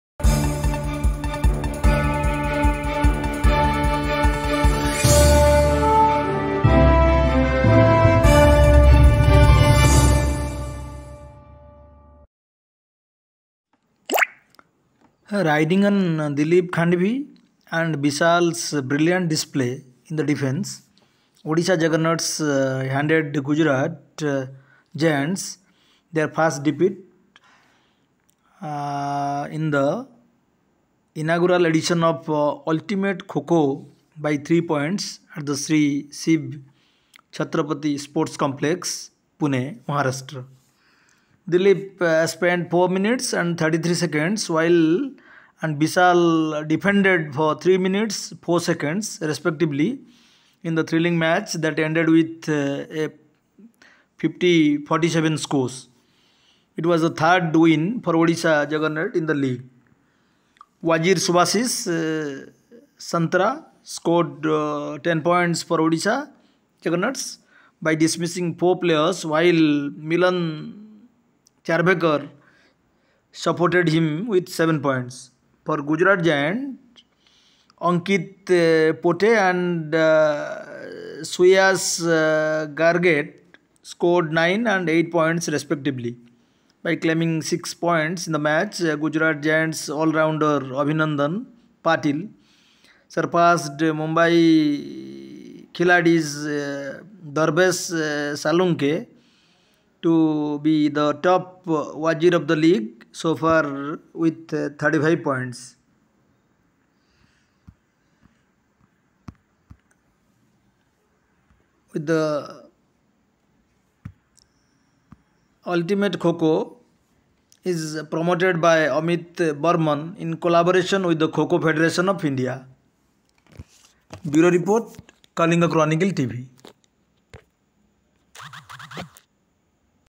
Ultimate Kho Kho: Dilip, Vishal heroics help Odisha Juggernauts sink Gujarat Giants by 3 Points
Kalinga Chronicle TV: People's Voice: Ultimate Kho Kho: Dilip, Vishal heroics help Odisha Juggernauts sink Gujarat Giants by 3 Points